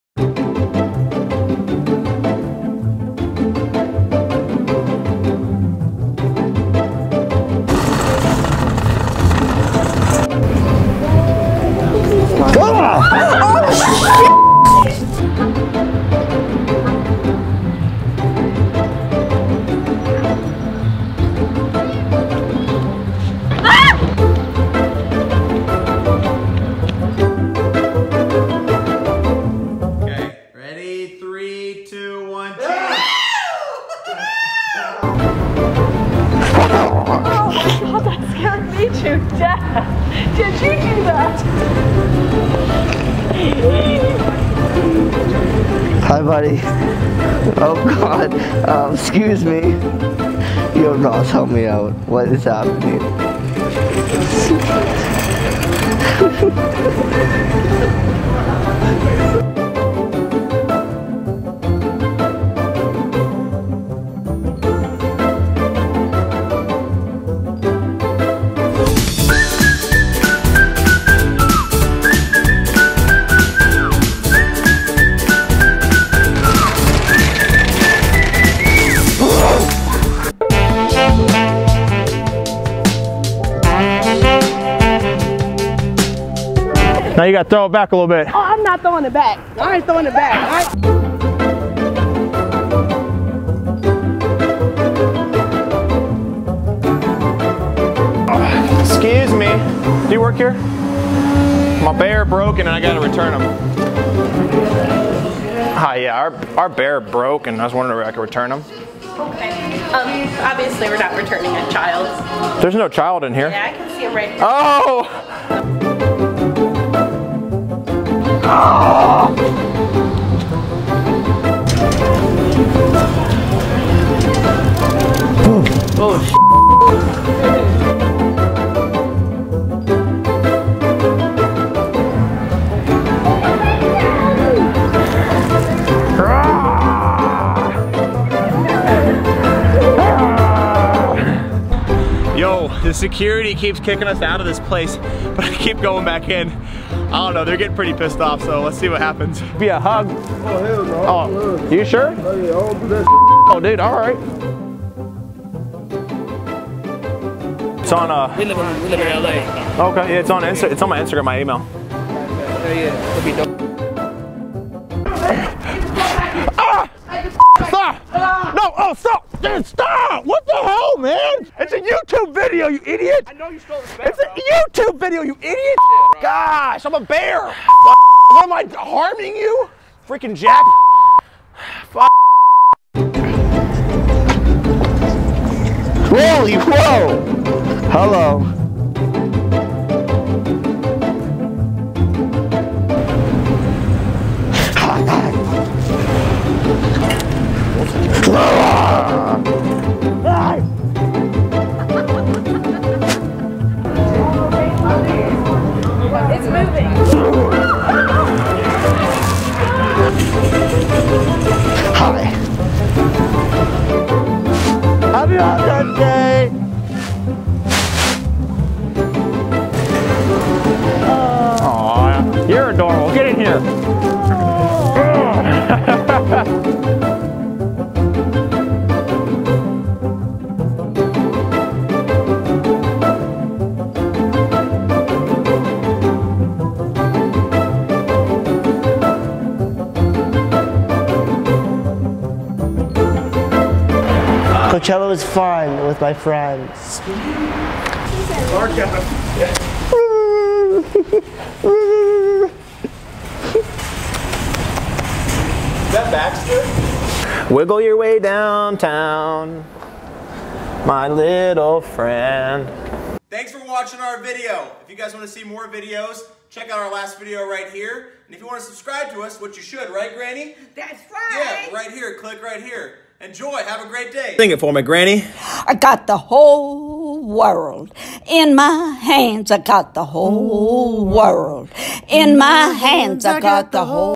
The Human Teddy Bear | Ross Smith
If a teddy bear came to life would you hug it or run?

In this prank, we had our friend dress up in a teddy bear outfit and surprise unexpecting people. He even managed to scare Granny!